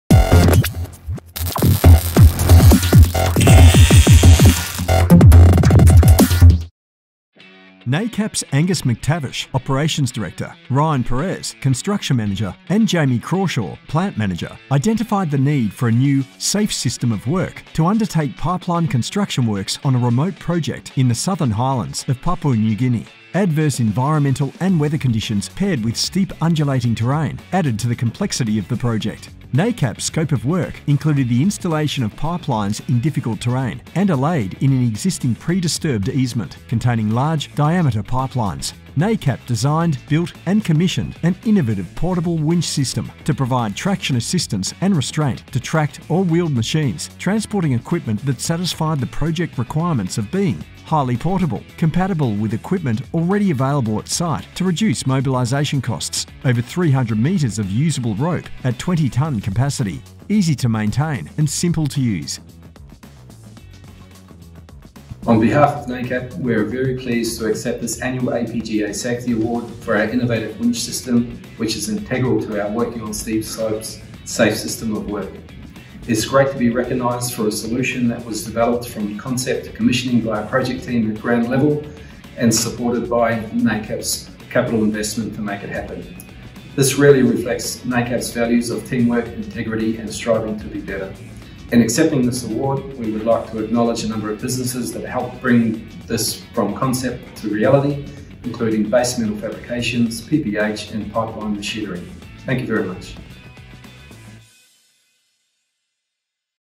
NACAP's APGA Safety Award - Excavator Winch
Nacap are very proud to have been awarded the 2021 APGA Safety Award for our excavator based portable winch system.

This safe system was developed to control the risk of transporting heavy equipment up and down steep slopes in harsh environmental and adverse weather conditions. What makes this acknowledgement pleasing is the fact that the project team on the ground recognised this risk and set about to design, build, test and commission an innovative and practical cost effective solution.

A special thanks to Angus McTavish (Operations Director), Ryan Perez (Construction Manager) and Jamie Crawshaw (Plant Manager) who were instrumental in recognising the challenge and delivering a successful solution.

Nacap would also like to acknowledge and thank Degra, Base Metal Fabrications, PPH and PLM for their involvement in this process.